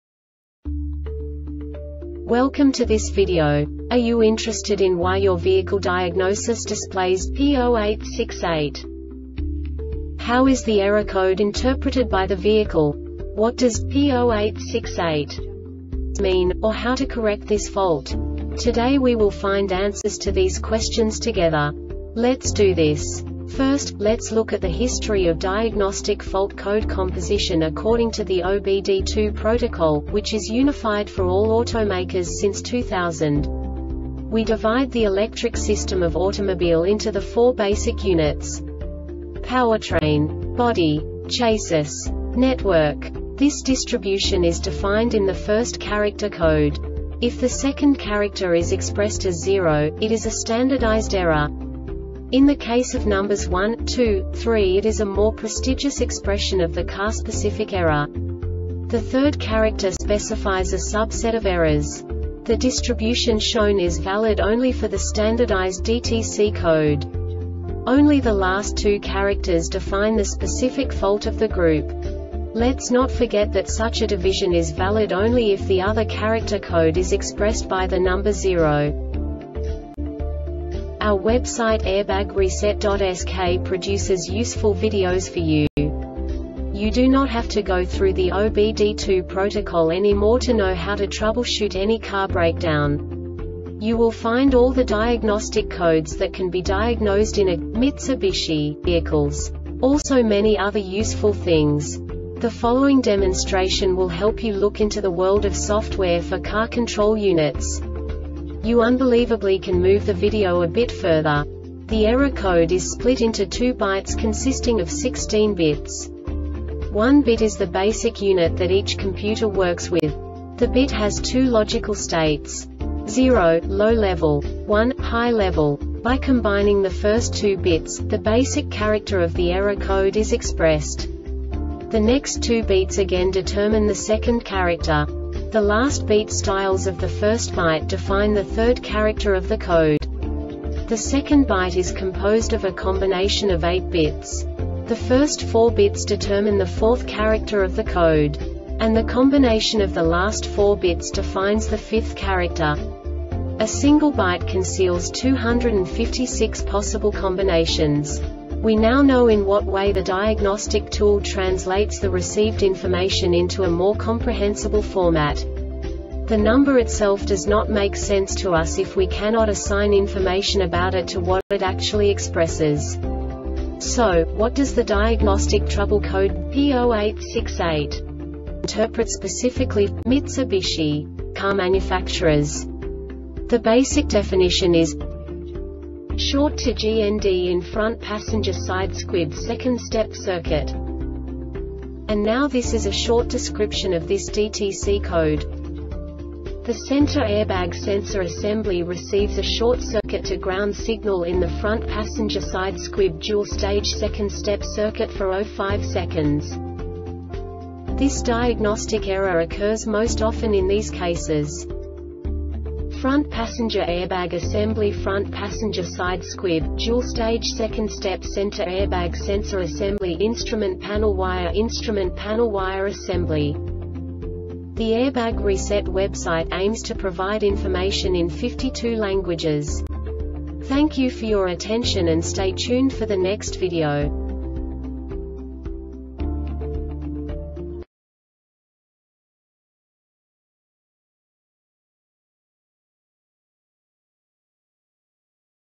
DTC Mitsubishi P0868 Short Explanation
Contents:
0:21 Basic DTC analysis according to OBD2 protocol standard.
1:48 Insight into programming
2:58 A diagnostically understandable description of the Diagnostic Trouble Code
3:05 Basic error definition

Make:
Mitsubishi
Code:
P0868
Definition:
Short to GND in Front Passenger Side Squib 2nd Step Circuit
Description:
The center airbag sensor assembly receives a short circuit to ground signal in the front passenger side squib (dual stage - 2nd step) circuit for 0.5 seconds.
Cause:
 Front passenger airbag assembly (front passenger side squib,dual stage - 2nd step) Center airbag sensor assembly Instrument panel wire Instrument panel wire assembly